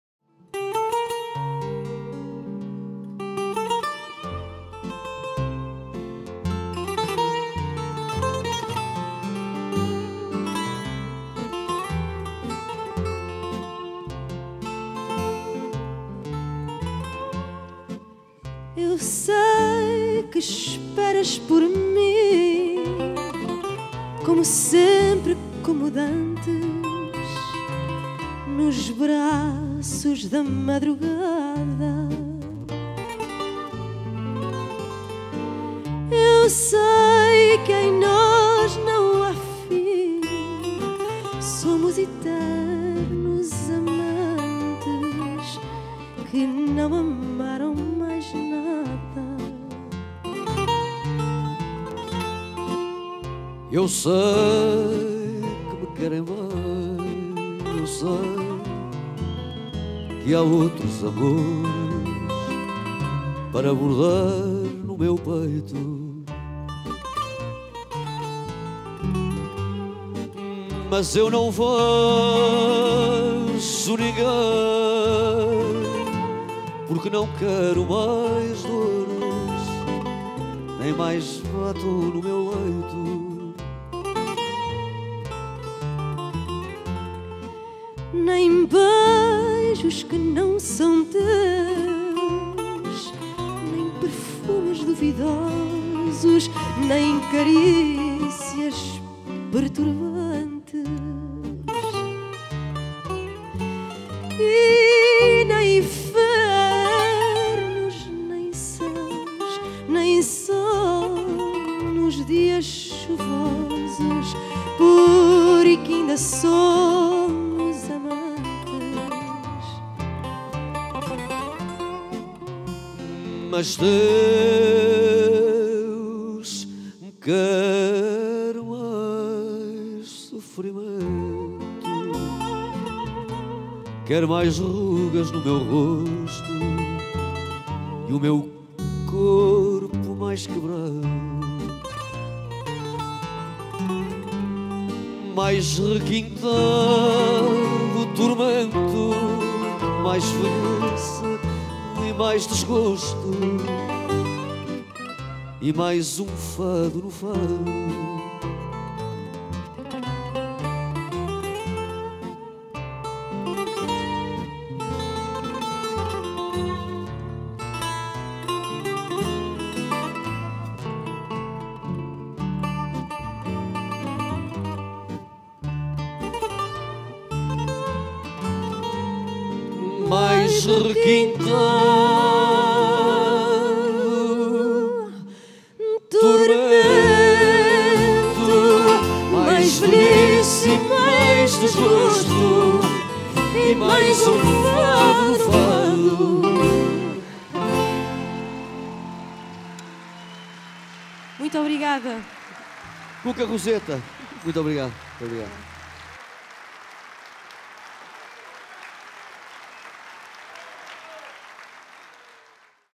Cuca Roseta & Camané
Cuca Roseta canta com Camané, em Matosinhos.